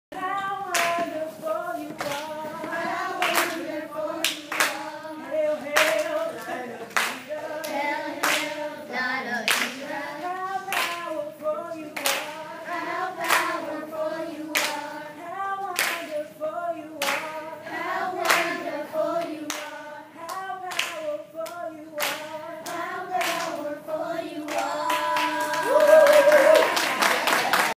Shalom Summer Campers 2011-III
Minister Erica Cole leads Shalom campers in afternoon program